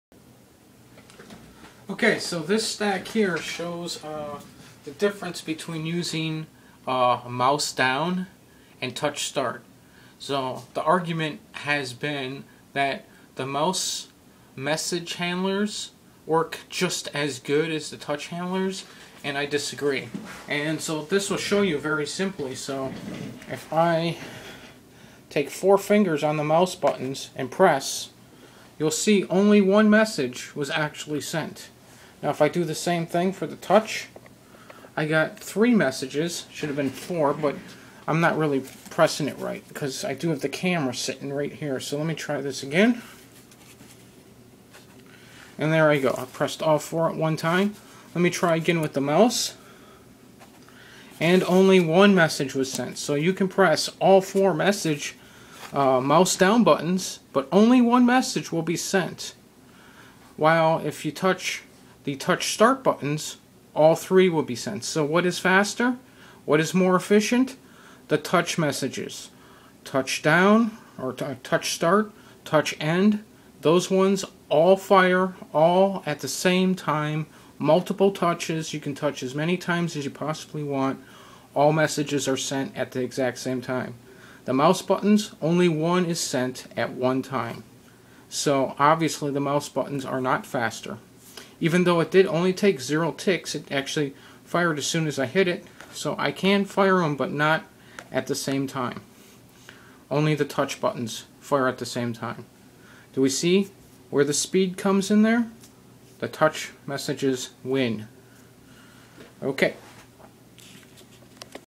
Livecode: touch vs mouse messages
Livecode mobile development supports touch syntax, but also allows for mouse syntax. Why? Is one faster and more efficient over the other? Yep the Touch messages are way more faster and allow for multiple of things to happen at one time.  This demo shows what happens, and why one should use touch syntax in mobile for tip top highly optimized and efficient end products.